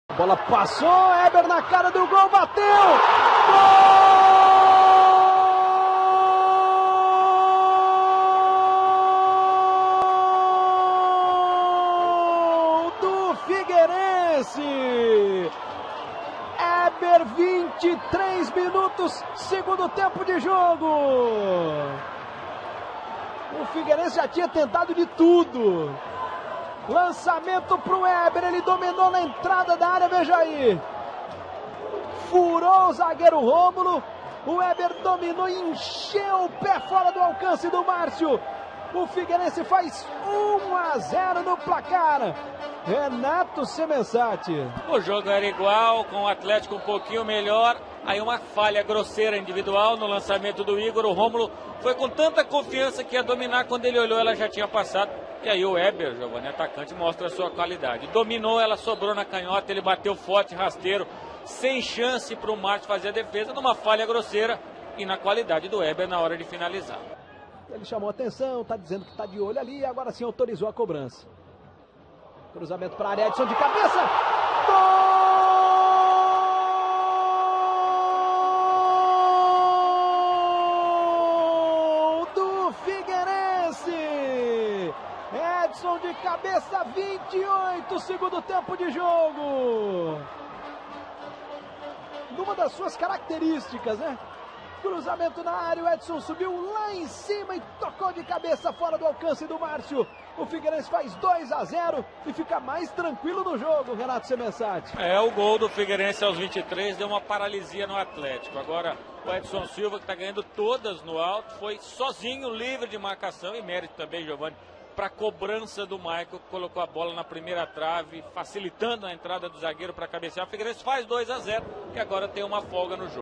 Figueirense 2x0 Atlético-GO - Brasileirão 2011 - 04/06/2011
FIGUEIRENSE 2 X 0 ATLÉTICO-GO
Wilson; Bruno, João Paulo, Édson Silva e Juninho; Ygor, Túlio, Maicon e Wellington Nem (Rhayner); Reinaldo (Héber) e Aloísio (Roger Carvalho).  Márcio; Adriano, Gílson, Anderson e Thiago Feltri; Agenor (Felipe Brisola), Pituca (Rômulo), Ramalho e Vítor Júnior; Felipe (Anselmo) e Marcão.
Técnico: Jorginho. Técnico: PC Gusmão.
Gols: Héber, aos 23, e Édson Silva, aos 28 minutos do segundo tempo.
Cartões amarelos: Rhayner (FIG), Agenor, Adriano e Pituca (ATG).
Estádio: Orlando Scarpelli, Florianópolis (SC). Data: 04/06/2011. Árbitro: Wilson Luiz Seneme (SP). Auxiliares: Alessandro Rocha de Matos (BA) e Emerson Augusto de Carvalho (SP).